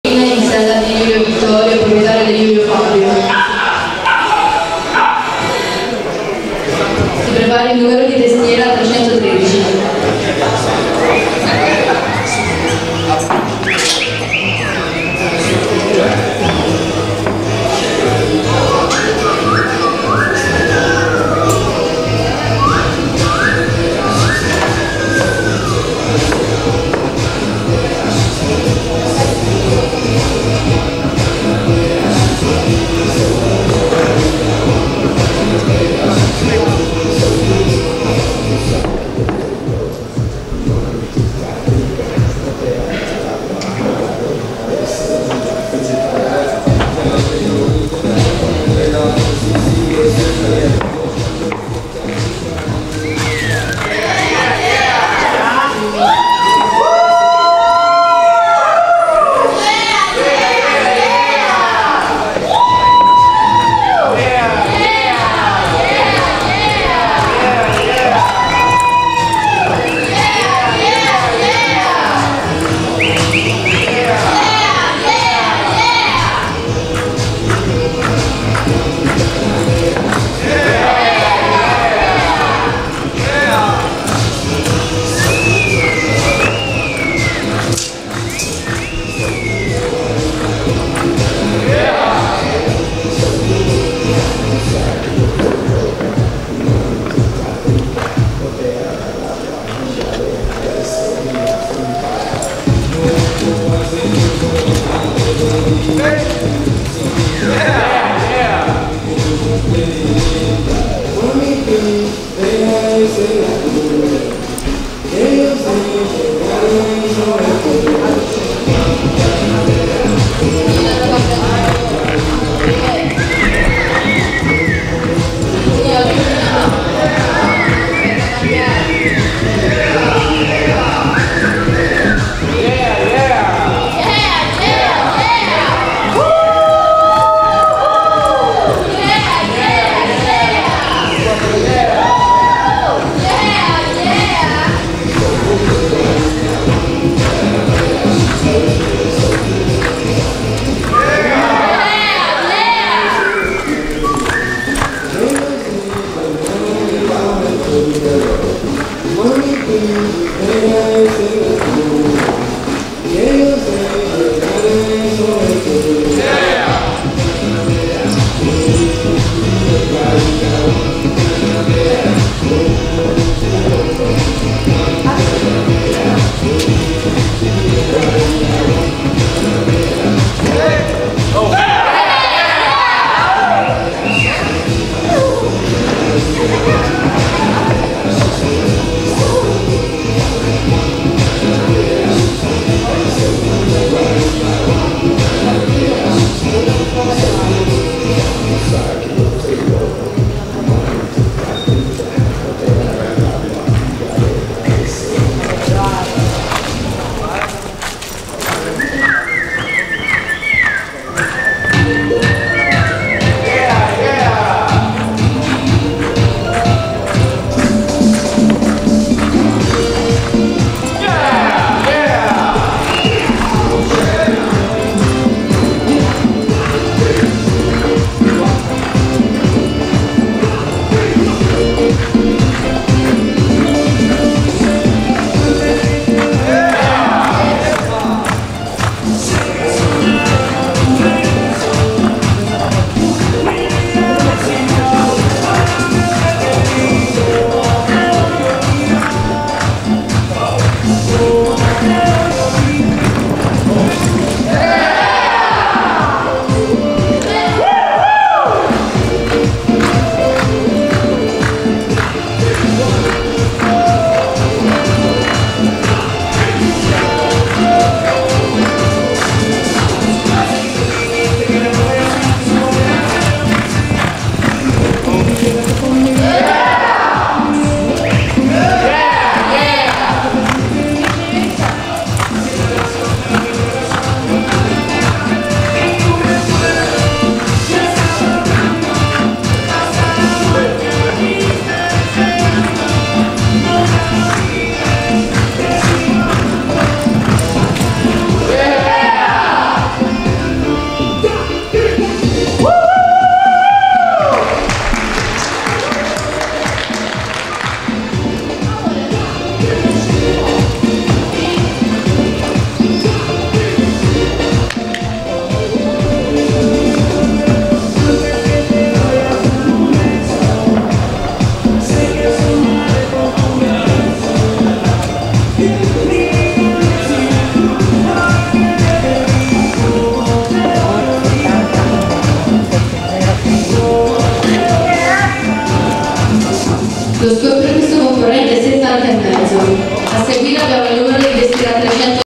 4 show ACCR 2024 - DE IULIO VITTORIO & DIDAGAIN score 70,5
DE IULIO VITTORIO & DIDAGAIN owner DE IULIO FABIO score 70,5 - ACCR-IRHA-FISE 3 YRS OLD OPEN TROPHY Champion - Team Run e 4 show ACCR-IRHA-FISE 2024 - credits Bonaga Communication